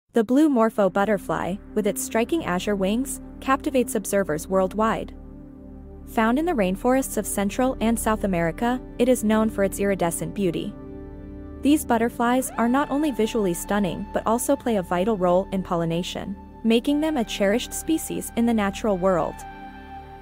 Dazzling Flight: The Enchanting Blue Morpho Butterfly
Immerse yourself in the captivating world of the Blue Morpho Butterfly as it gracefully flutters through the lush rainforests of Central and South America. Marvel at its vibrant blue iridescent wings, captivating in-flight maneuvers, and intriguing life cycle. This short video offers a glimpse into the magical beauty and wonder of one of nature's most stunning creatures.